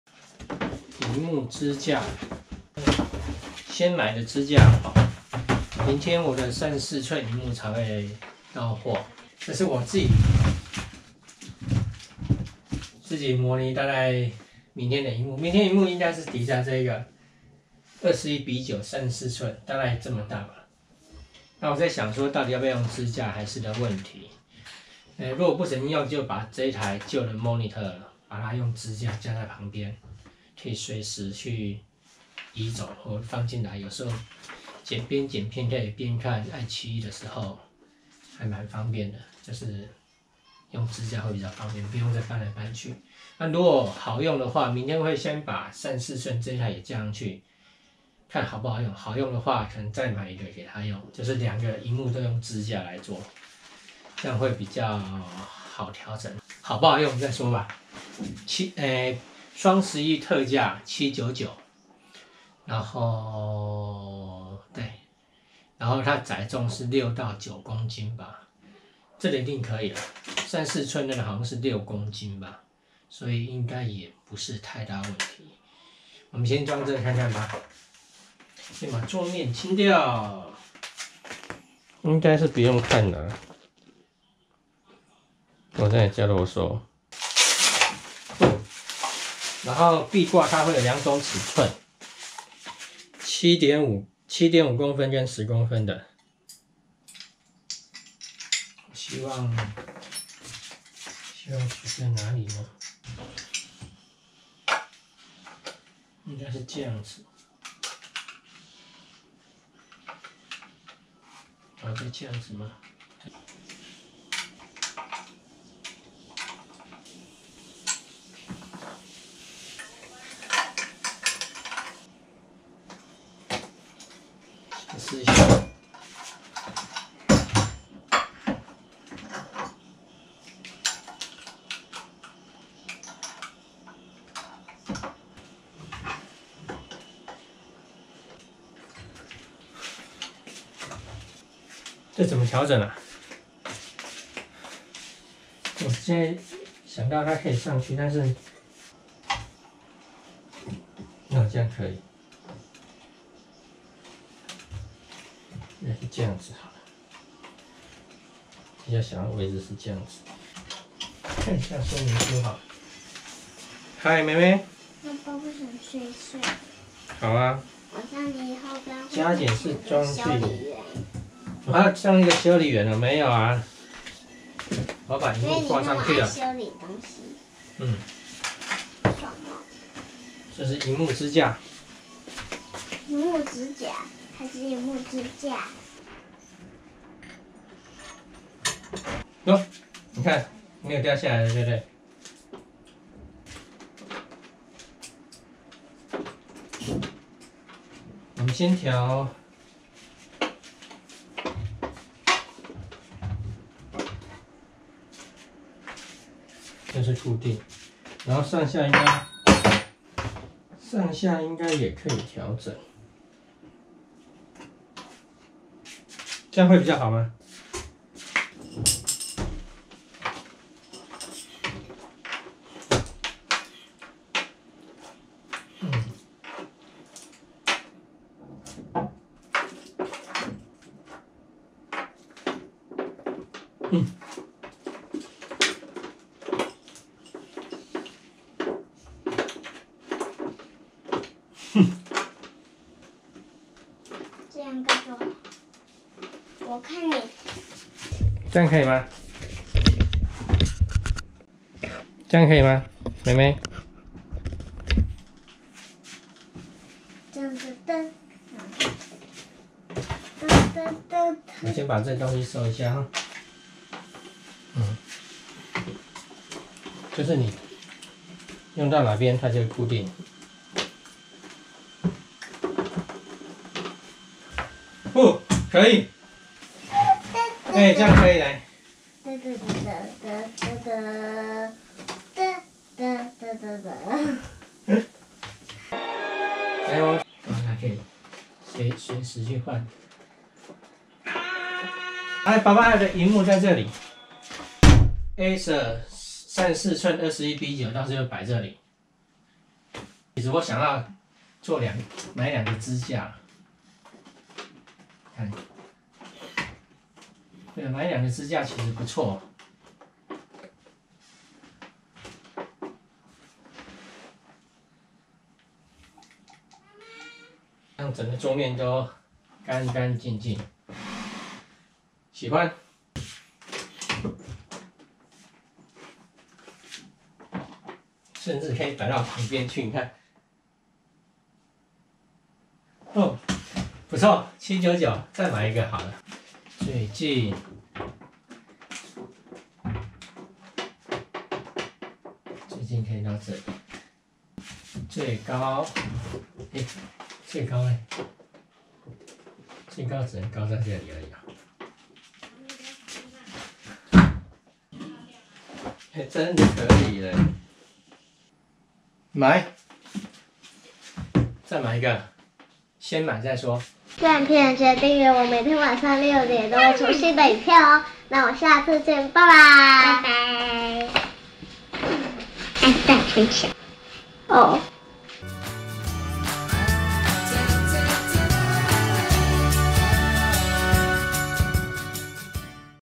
雙11開箱EP04_超好用的桌上型螢幕支架內建3.0USB_191113
原本只是買來裝裝看的螢幕支架,發現真的不錯用,趕緊再買一隻.